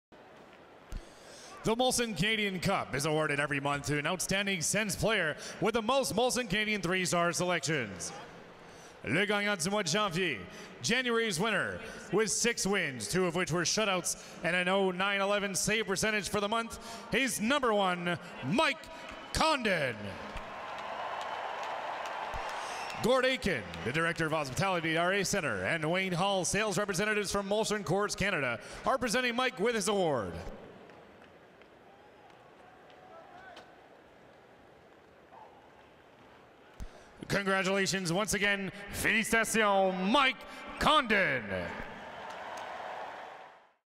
Feb 9: Molson Canadian Cup Winner - Mike Condon
Awarded to the most outstanding Sens player of the month, The Molson Canadian Cup winner for January is Mike Condon.